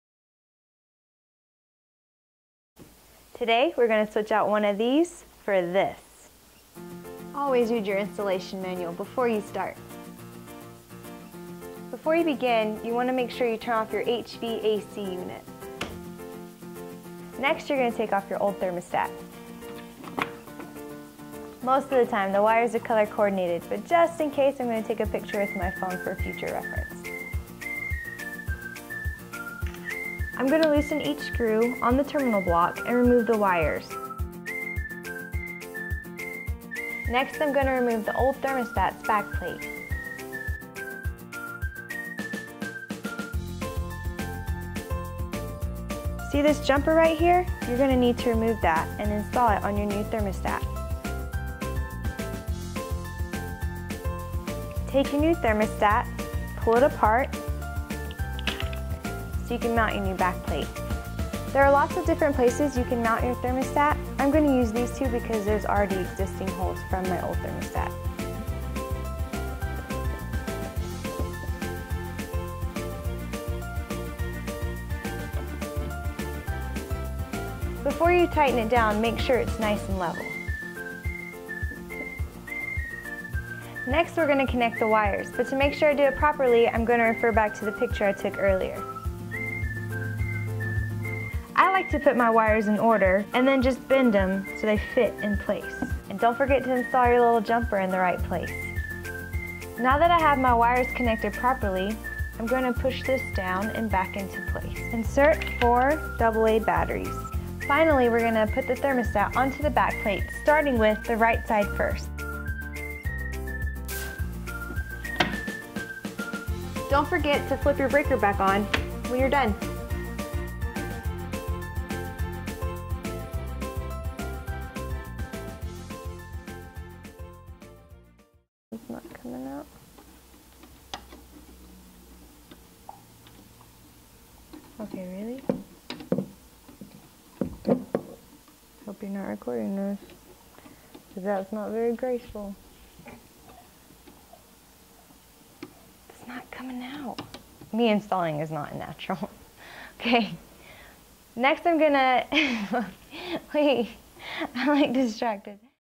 Qolsys IQ Panel : How to Install a Z-Wave Thermostat
How to Install a Z-Wave Thermostat for use in conjunction with your Qolsys IQ Wireless Security System. For the lowest No-Term alarm monitoring rates contact GEOARM Security.